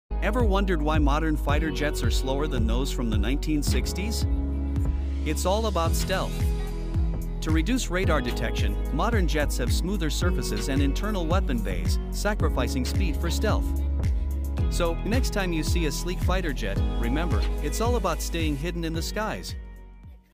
Why Are Modern Fighter Jets Slower?
Have you ever wondered why modern fighter jets might not always fly at their maximum speeds? Join us as we unravel the intriguing reasons behind this phenomenon. Explore the intricate balance between speed, stealth, maneuverability, and mission requirements that influence the performance of today's cutting-edge fighter aircraft. Discover how factors like stealth design, operational considerations, and technological advancements play a role in shaping the speed capabilities of modern jets. Gain insights into the fascinating world of aviation as we delve into why speed isn't the only measure of a fighter jet's prowess.